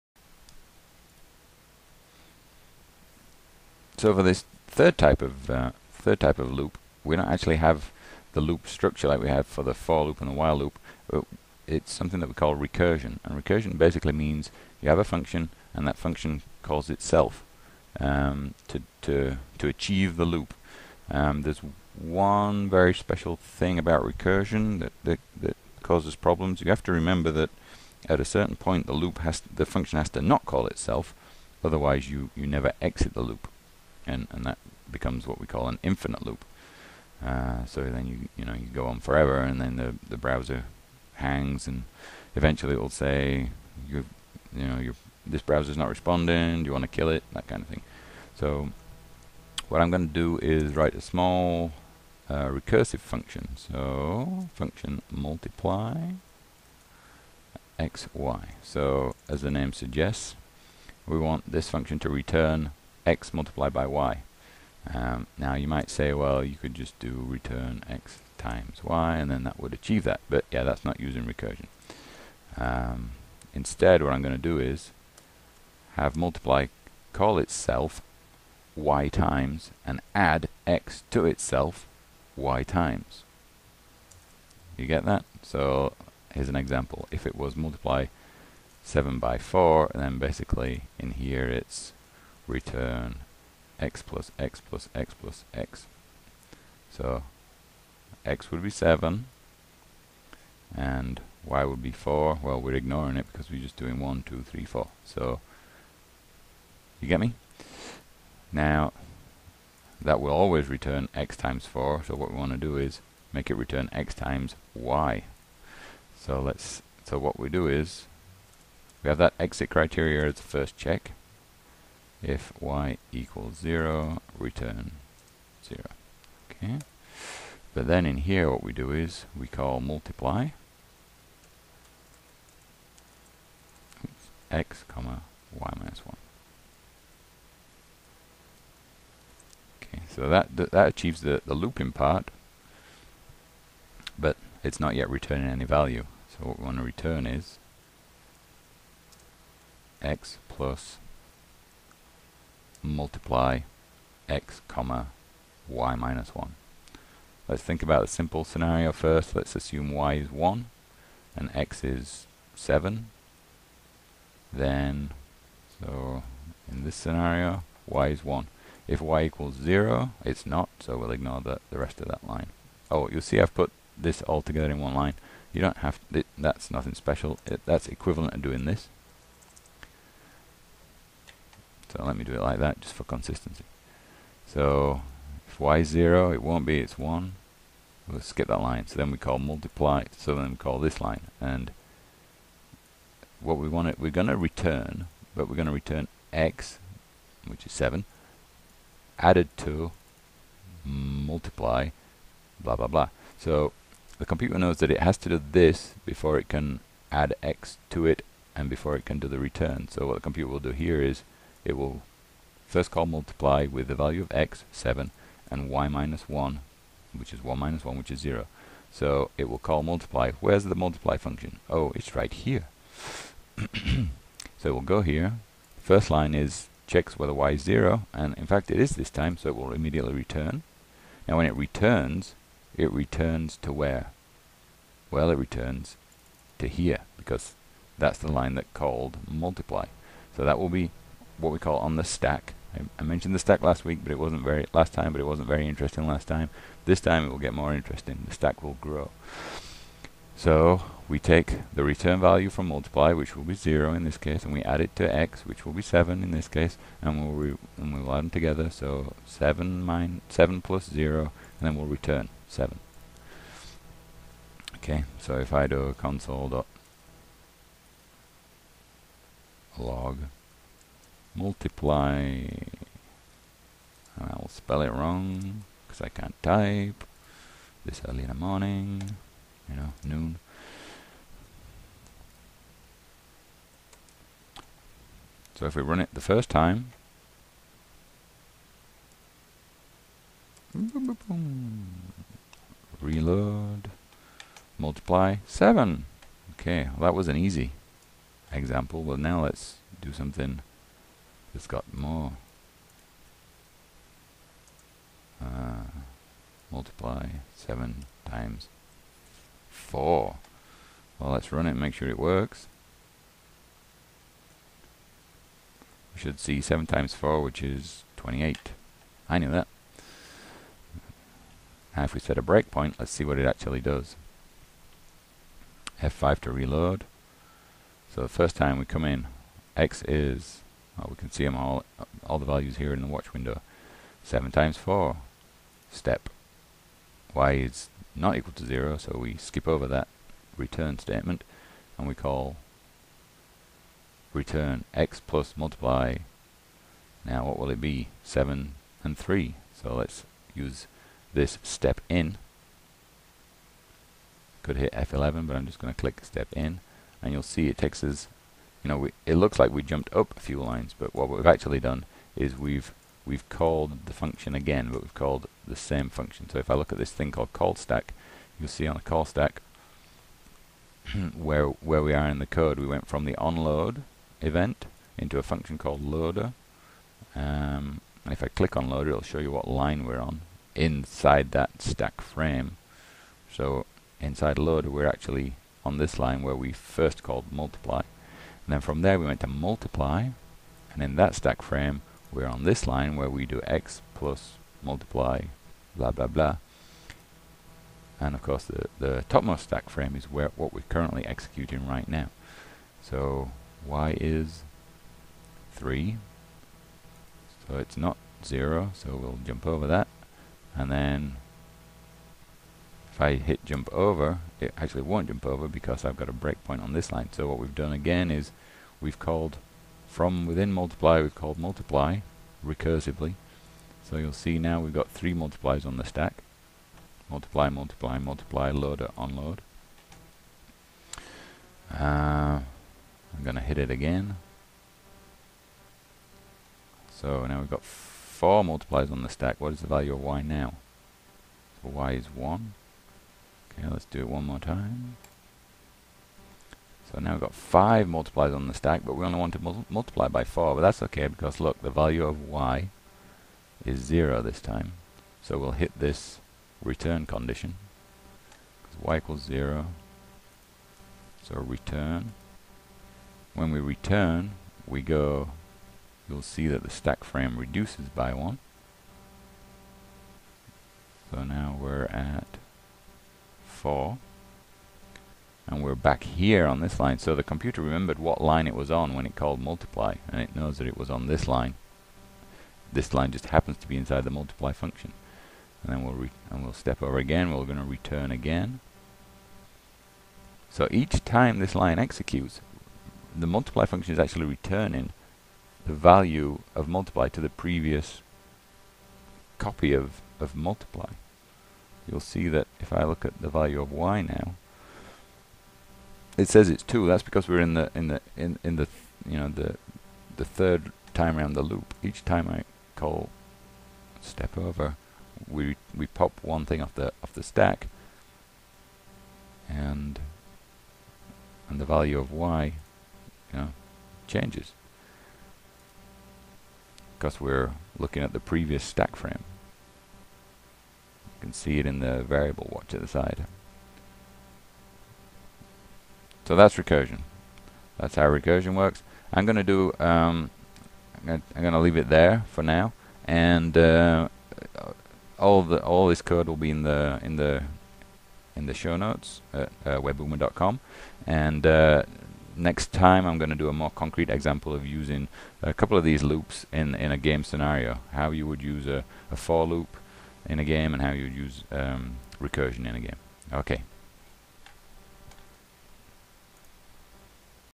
Game Coder Episode 005 - part 2
LOOPS - RECURSION

Going around in circles! - Looping in software.

This time we look at the ways we make our code loop, so that we run a piece of code multiple times.

Loops are what we use when we want to do something repeatedly. Say we need to print something on the screen more than once, or play a sound over and over, until the user responds to us.

As is often the case with programming, there's more than one way to do a loop in software. In this episode, we look at three common ways to loop:

    - for-loop,
    - while-loop, and
    - recursion